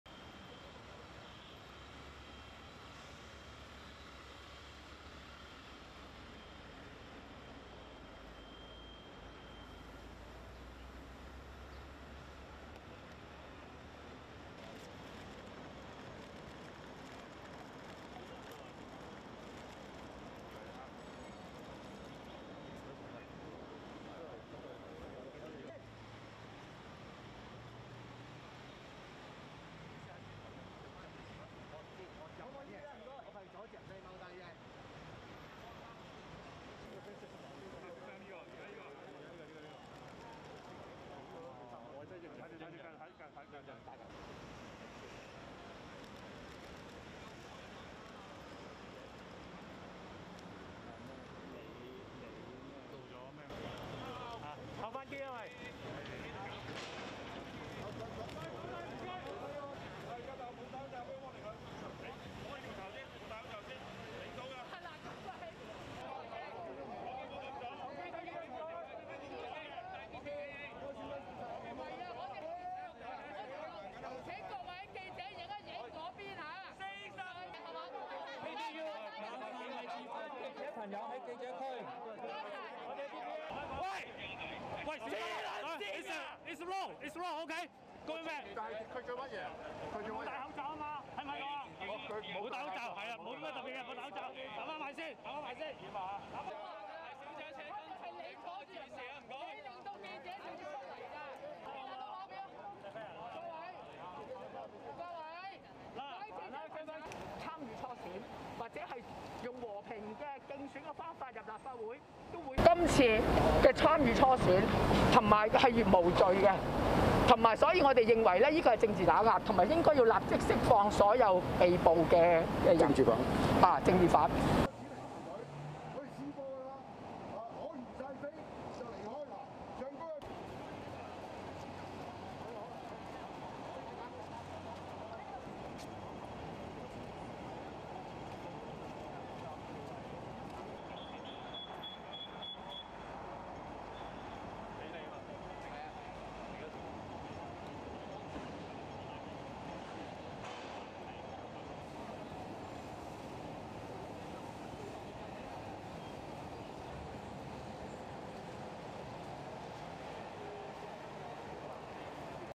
Trial of activists begins in Hong Kong
RESTRICTION SUMMARY:

ASSOCIATED PRESS
Hong Kong  - 6 February 2023
1. Various of prison van carrying defendants driving into court building
2. Wide of defendant Lau Wai-chung walking into court
3. Wide of defendant Helena Wong walking into court
4. Wide of defendant Cheng Tat-hung walking into court
5. Wide of defendant Lee Yue-shun walking into court
6. Wide pan from journalists to protesters
7. Various of police telling protesters to move away
8. Wide of a protester (male in dark green mask and glasses) being taken away and he was shouting at police using strong language in Cantonese
9. Various of protesters surrounded by police and journalists
10. Wide pan from journalists to protesters
11. SOUNDBITE (Cantonese) Chan Po Ying, activist and member of League of Social Democrats:
“Primary election is not crime. This is political repression. We demand the immediate release of all political prisoners.”
12. Wide of protest
13. Various of queue outside court
14. Mid of police
15. Close of a police dog
16. Close of court security guards and judiciary cordon
17. Wide pan from police to queue outside court
STORYLINE:
A national security trial started in Hong Kong on Monday for 18 of the 47 activists charged with “conspiracy to commit subversion” with some of them being detained without bail for nearly two years.

The 47 pro-democracy figures were arrested in 2021 over their involvement in an unofficial primary election that authorities said was a plot to paralyze Hong Kong’s government. Their arrests were widely seen as part of Beijing's crackdown on the Hong Kong's pro-democracy movement after massive protests in 2019.

Outside the West Kowloon Magistrates’ Court, three members of the League of Social Democrats organized a small protest. One member was taken away by the police for violating mask mandates when he took his mask off and tried to speak loudly.

Activists Chan Po-ying, who is the wife of one of the defendants Leung Kwok-hung, urged the immediate release of all political prisoners.

The trial is expected to last for 90 days.

AP video shot by Alice Fung